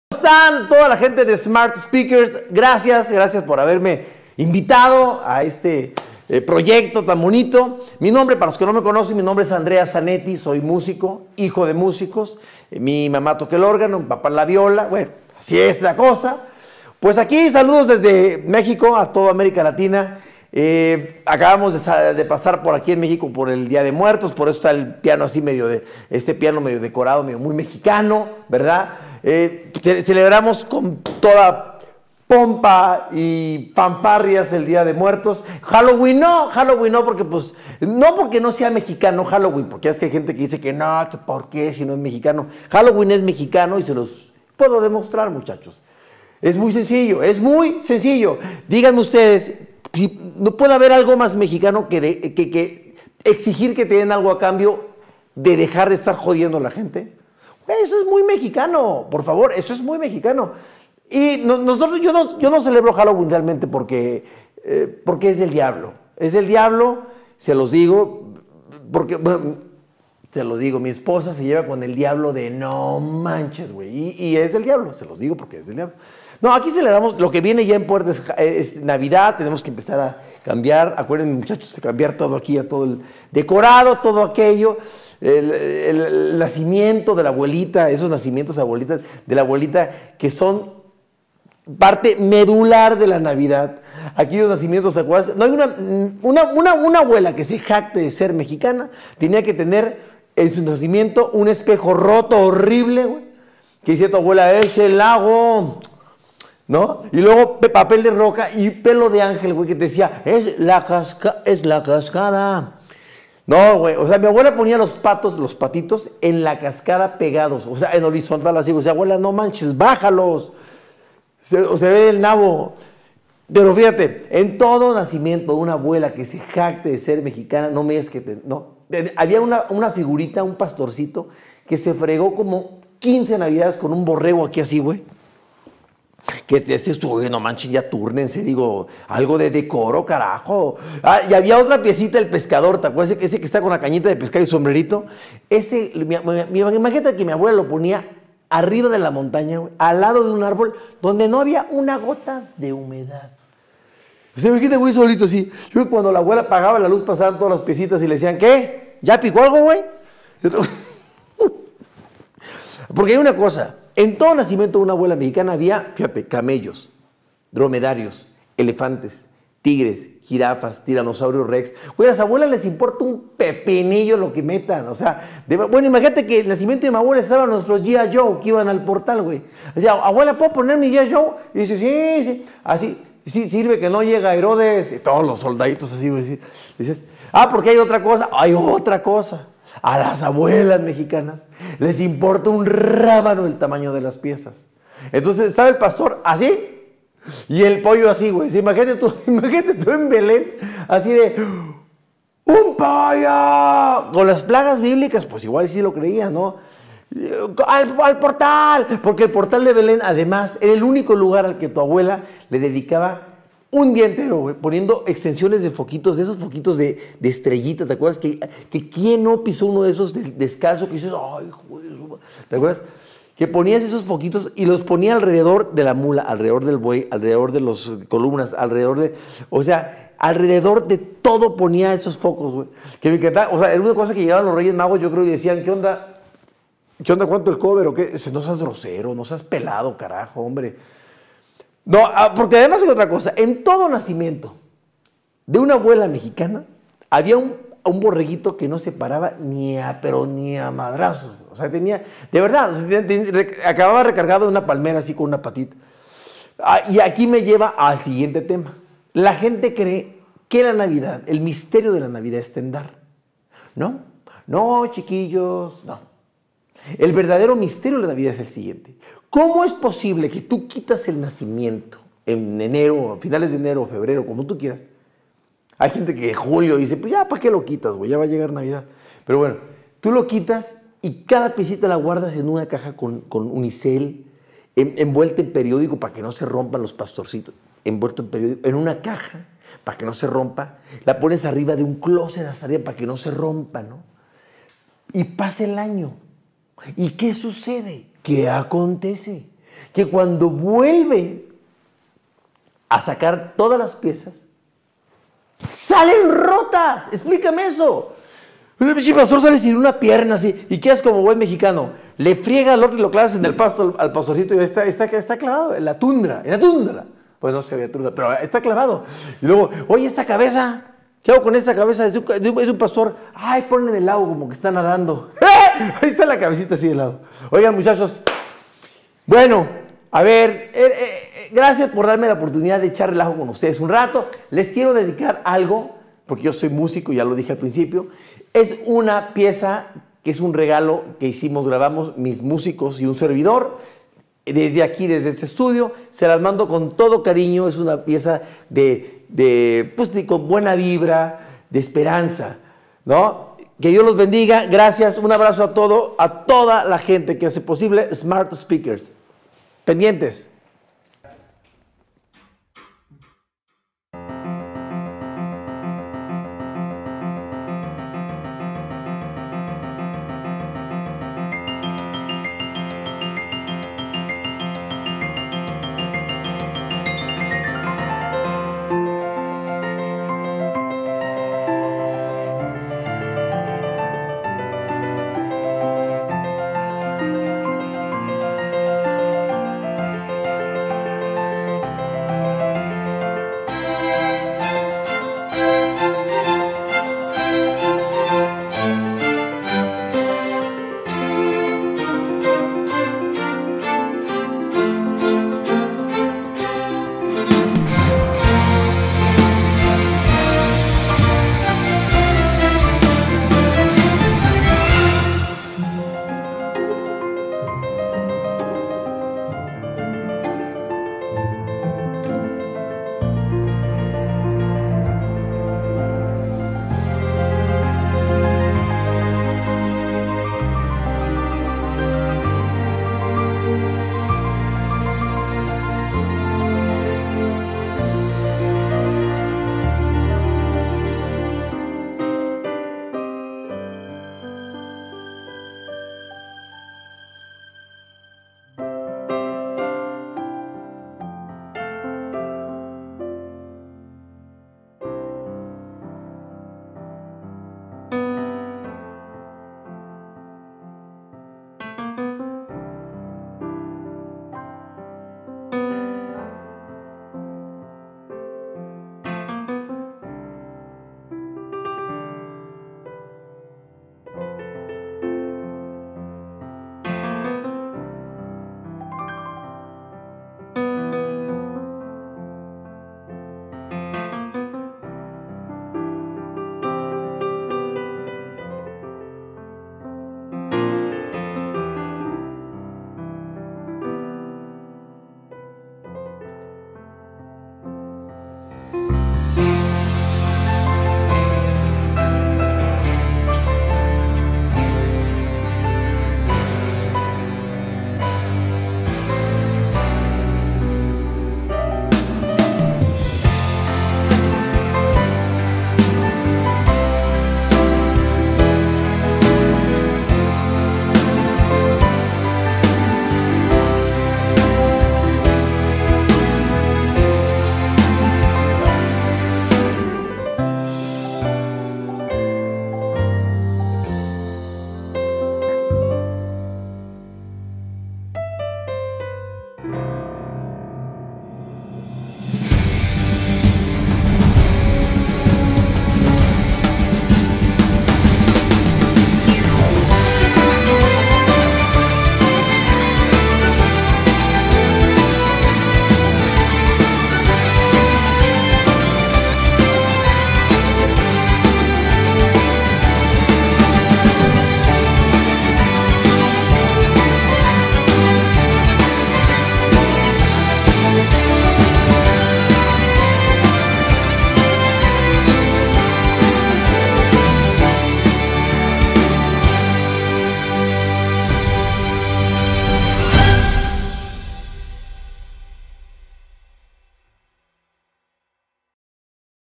Andreas Zanetti - Festival Smart Speakers
El divertido y carismático Andreas Zanetti fue parte del festival de Fin de año de Smart Speakers, ve su participación y la alegría de la música que trajo a los espectadores.